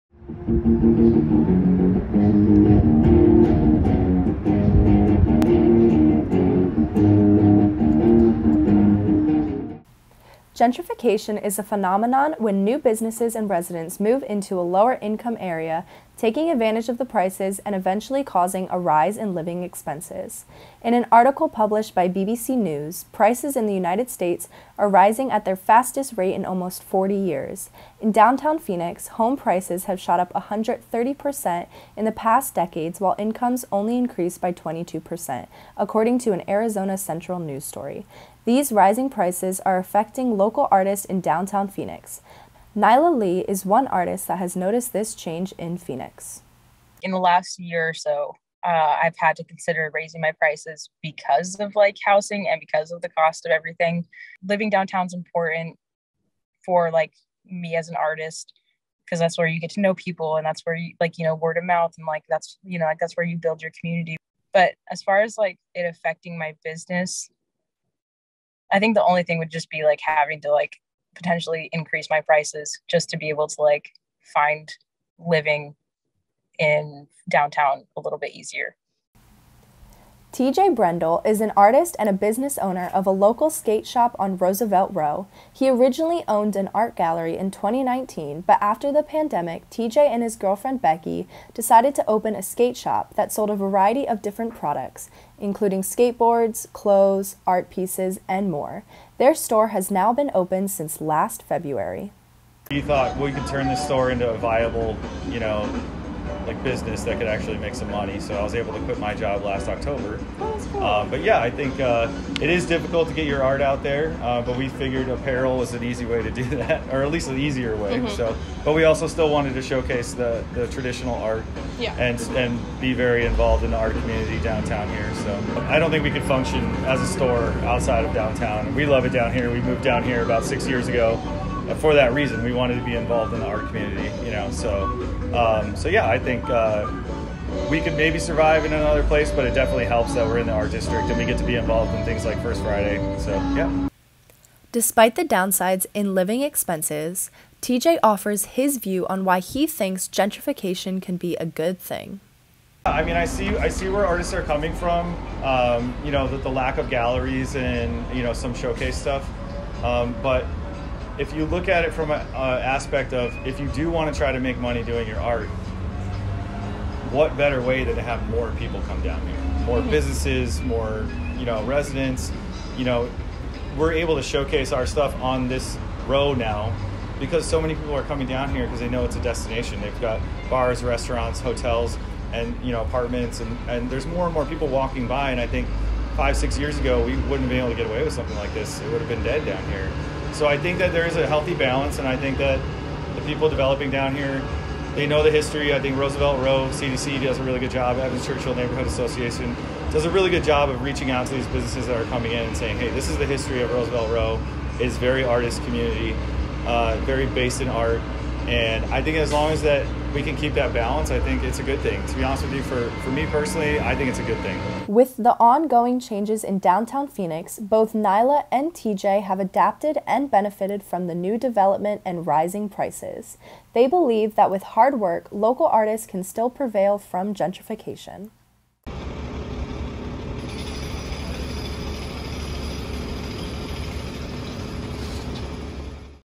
Phoenix Artists News Feature
This video discusses gentrification in Phoenix and how it is affecting local artists. Nyla Lee and TJ Brendle spoke about their personal experience as an artist in Phoenix.